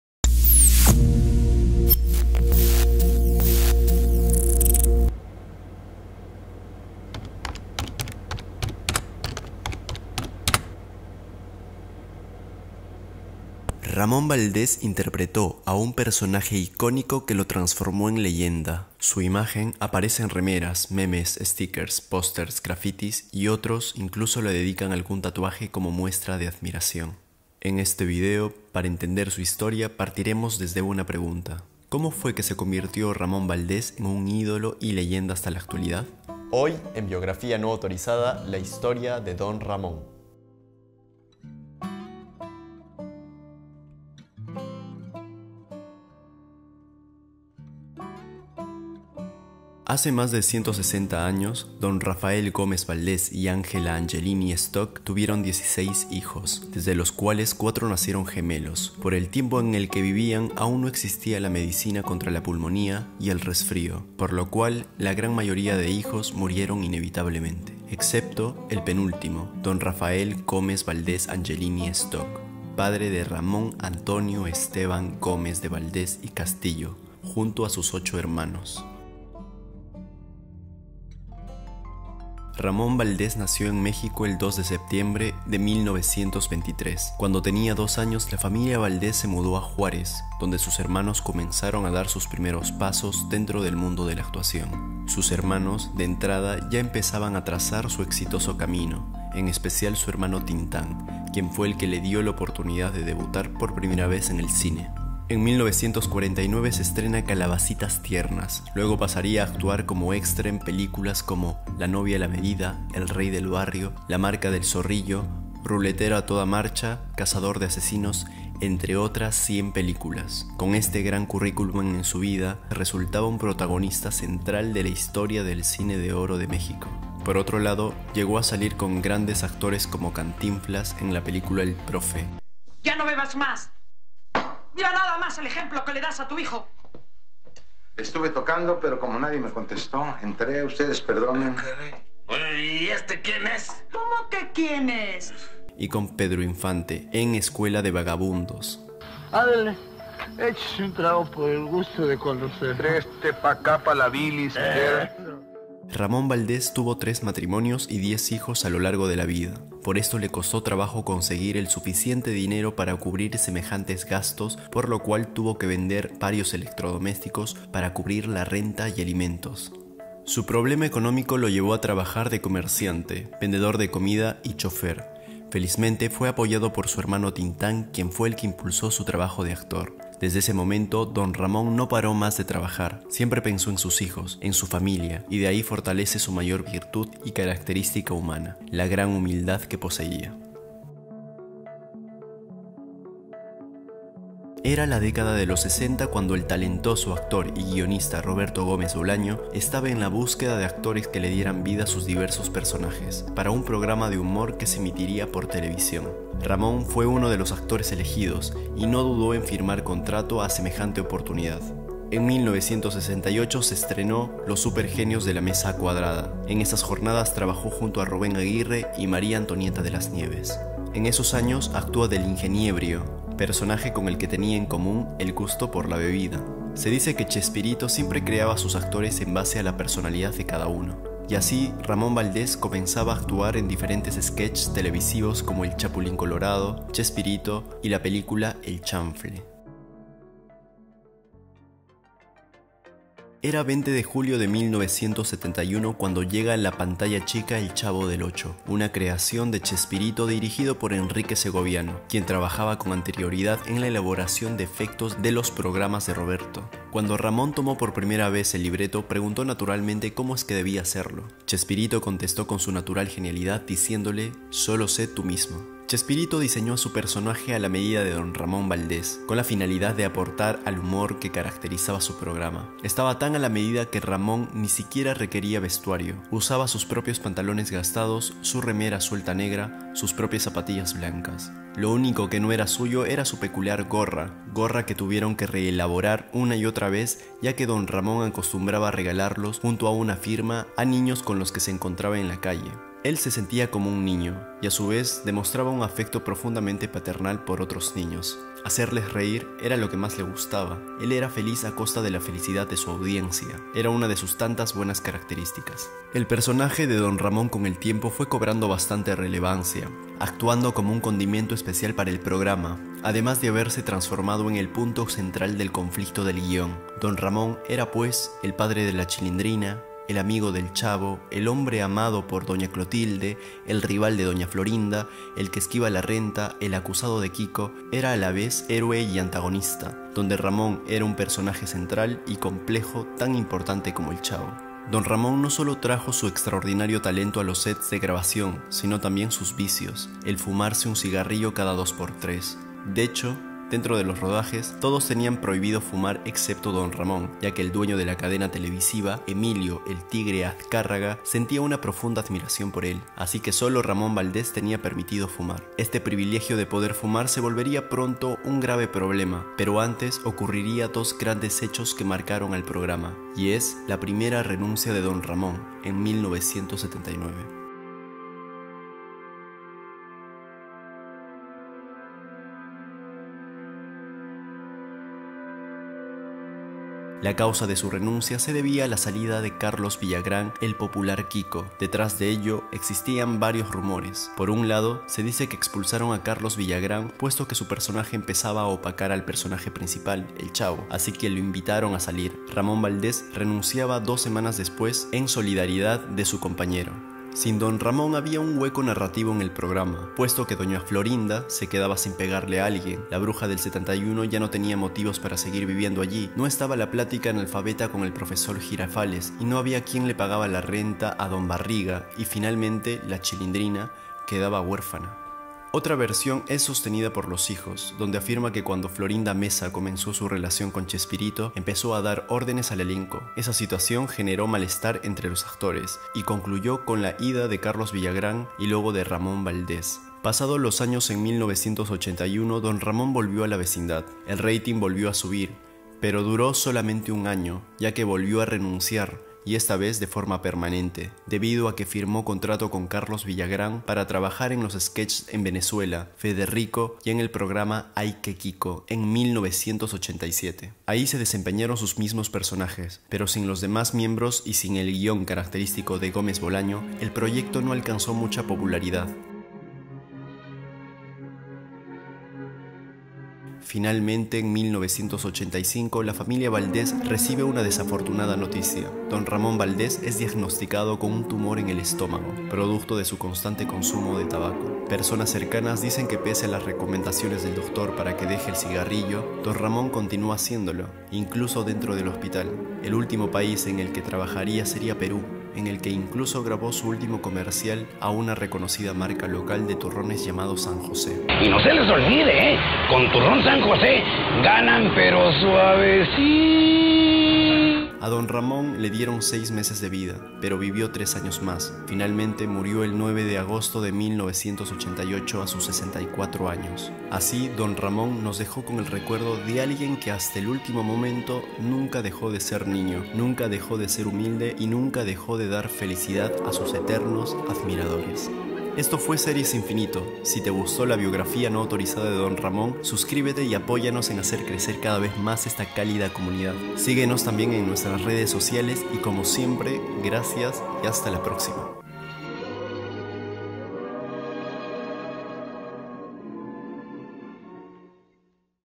La triste final de RAMÓN VALDÉZ | Biografía no autorizada #02
Ramón Valdéz, leyenda en Mexico y otros paises de Latinoamerica, tuvo una vida bastante alborotada e increible. En este episodio de BiografiaNoAutorizada, te contamos todos los detalles.